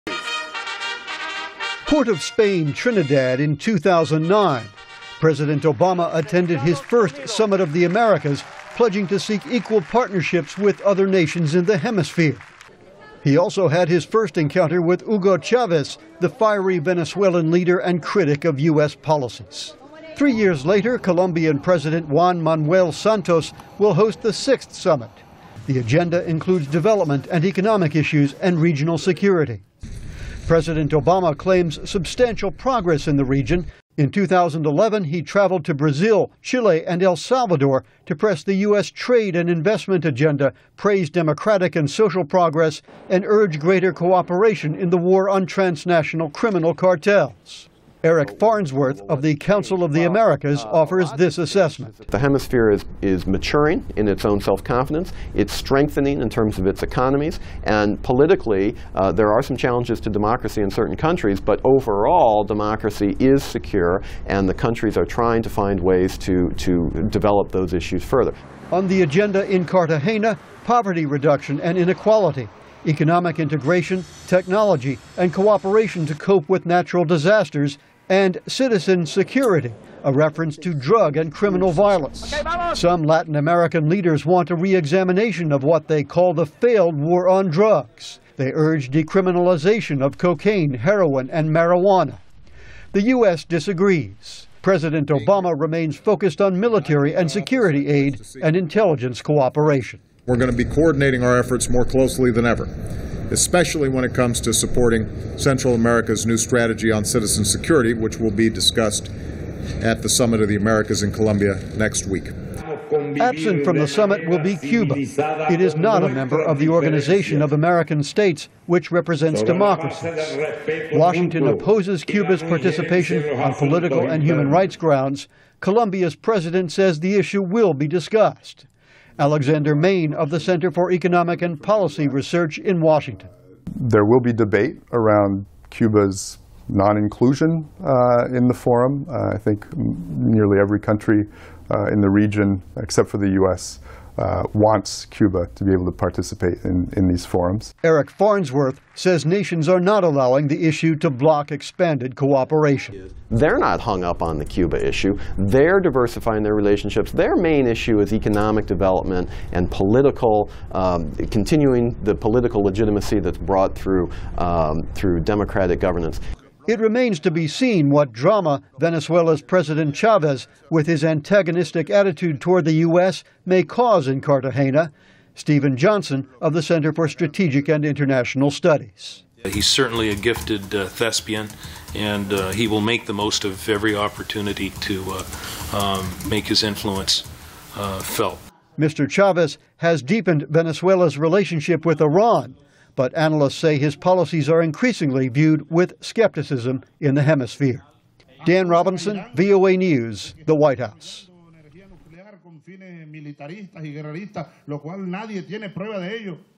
Drug Crime, Development Issues on Agenda for Summit of the Americas
President Barack Obama departs Washington on Friday for Cartagena, Colombia, where he will participate in the sixth Summit of the Americas.  VOA Senior White House Correspondent Dan Robinson reports.